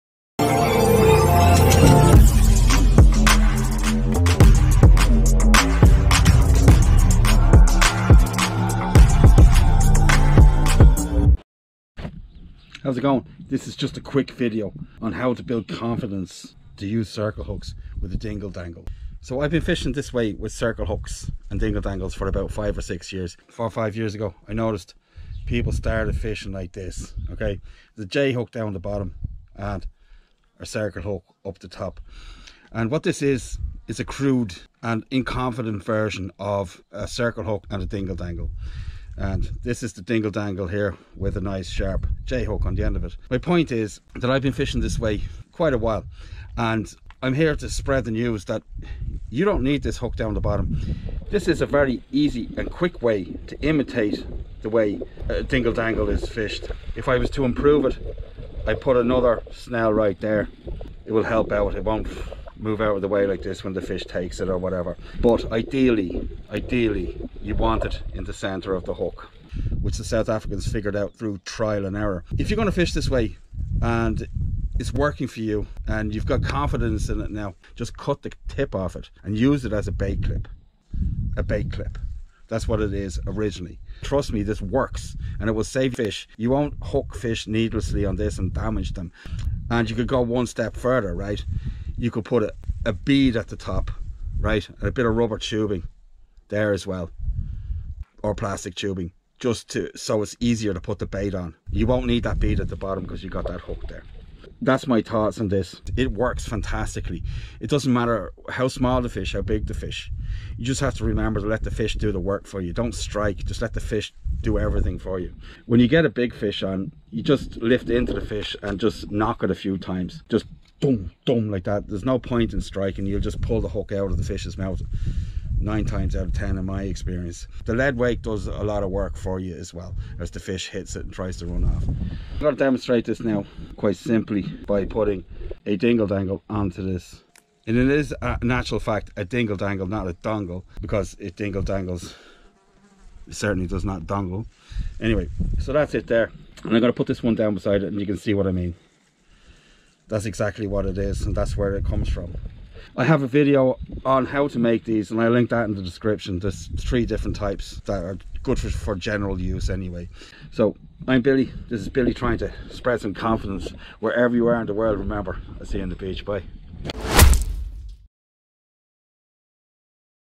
Building confidence, to catch more fish!
Quick and easy methods to help you catch more, and damage less fish